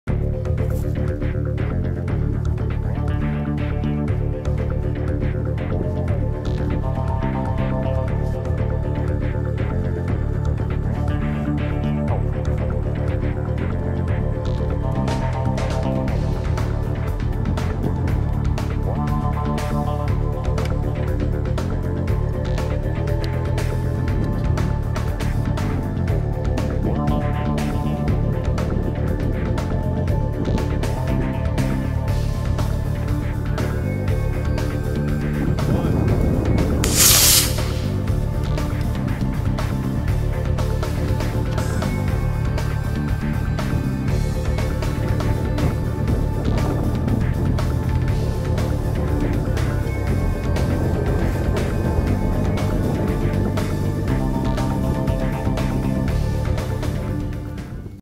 Estes Sky Hawker Launch
A rocket that will prey on the clouds, this slick blue bird soars over 1100 feet! No finishing or assembly is required for the Sky Hawker as he comes ready to fly right out of the box. This means more flying time for you!